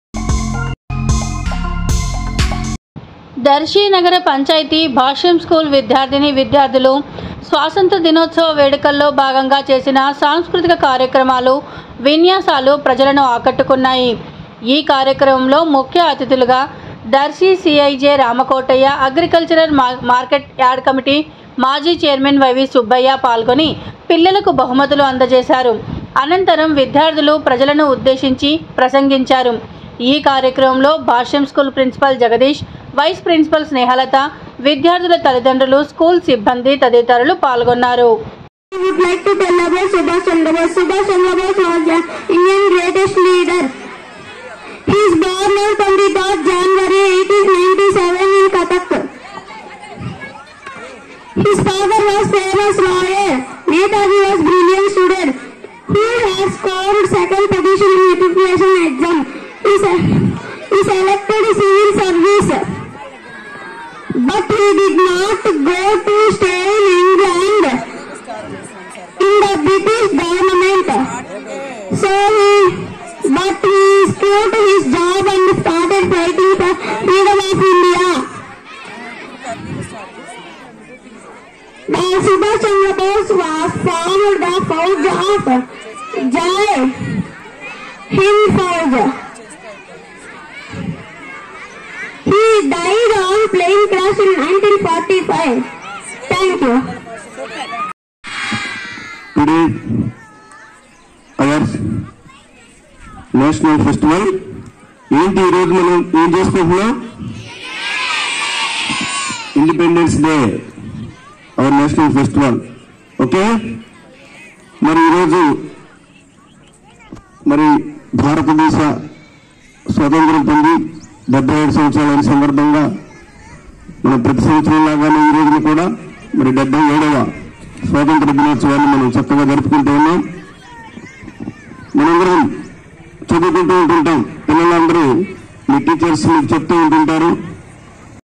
దర్శి నగర పంచాయతీ భాష్యం స్కూల్ లో 77వ స్వాతంత్ర దినోత్సవ వేడుకలు
దర్శి నగర పంచాయతీ భాష్యం స్కూల్ విద్యార్థిని విద్యార్థులు స్వాతంత్ర దినోత్సవ వేడుకల్లో భాగంగా చేసిన సాంస్కృతిక కార్యక్రమాలు విన్యాసాలు ప్రజలను ఆకట్టుకున్నాయి